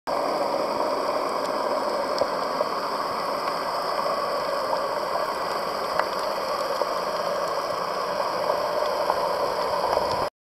Gallows Point Sea Turtle
Swimming with a sea turtle off of Gallows Point, St. John, USVI